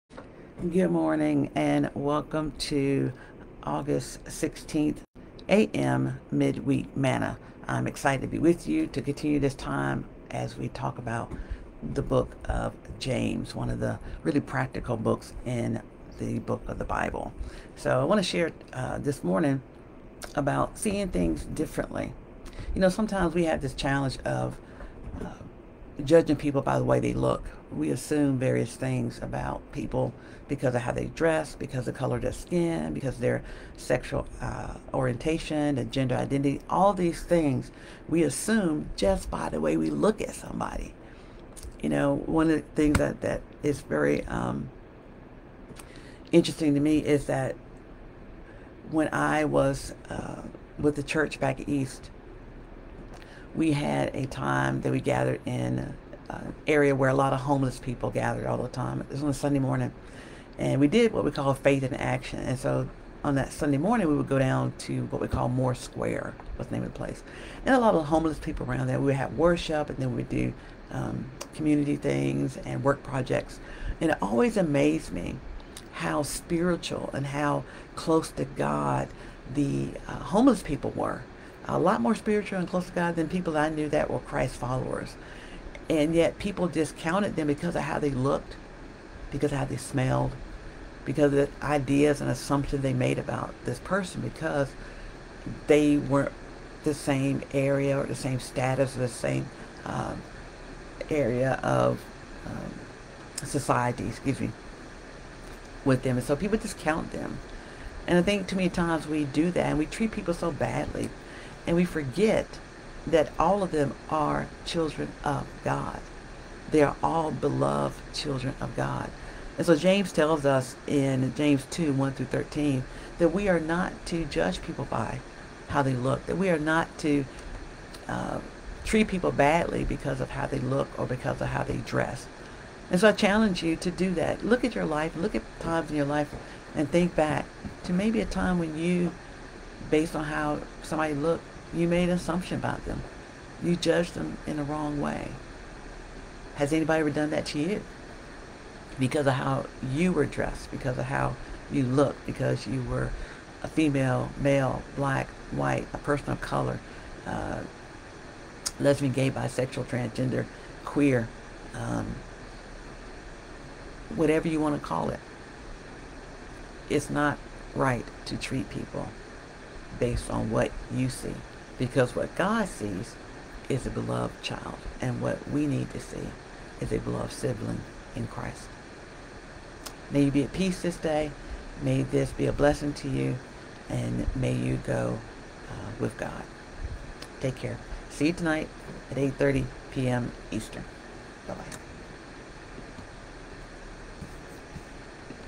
AM Midweek Manna 8/15 The Book of James
James tells us we are not to judge others by the way they look.  May this be a blessing to you.